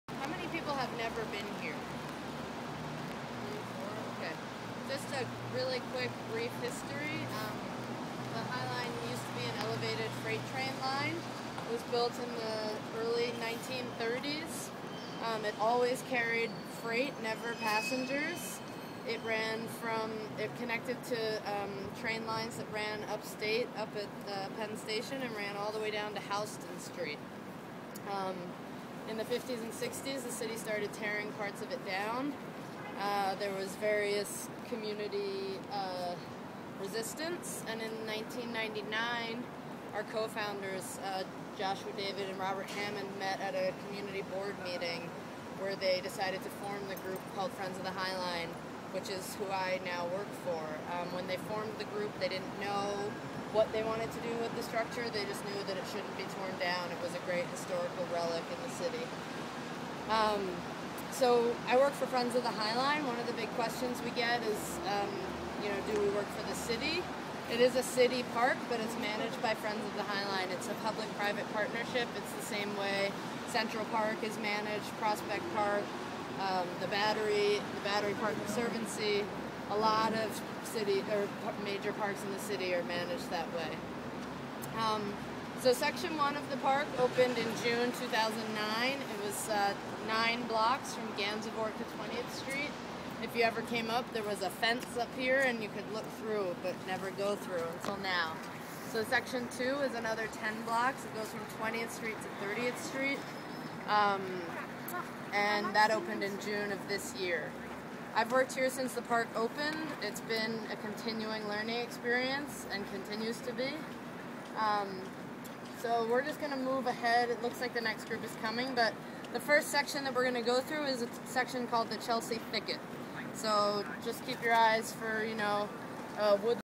2011-10-03_HL2tour01-intro.MOV
Last week I had the great fortune of touring The High Line; Section Two, with the Metro Hort Group. It was a little rainy and I was under the weather already but I enjoyed the informative talk and learned quite a bit from my groups tour guide Meave Turner, who is employed by Friends of the High Line as a gardener since the before the park opened in 2009.
I would also like to thank Patricia Jonas for organizing these Metro Hort fieldtrips.